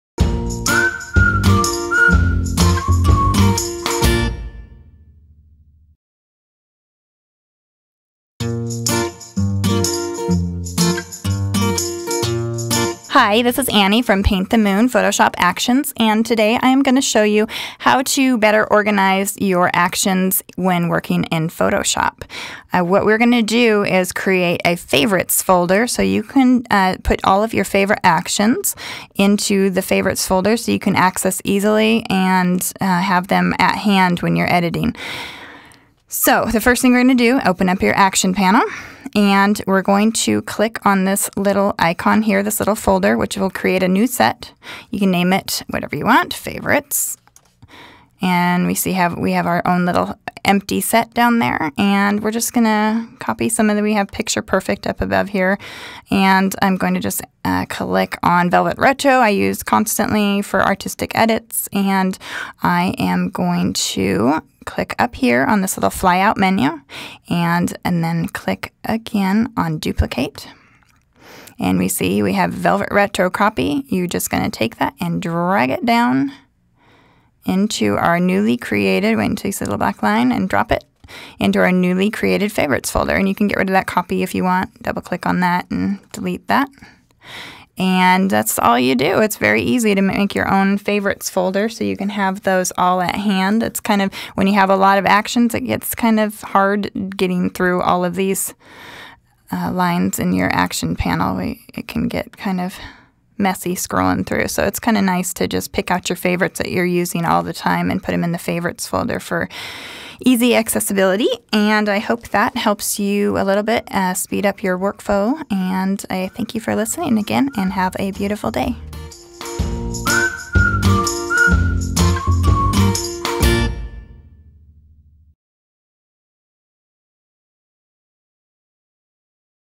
Organizing Your Actions in Photoshop by Paint the Moon
How to organize your actions in Photoshop within your actions panel. By Paint the Moon Photoshop Actions.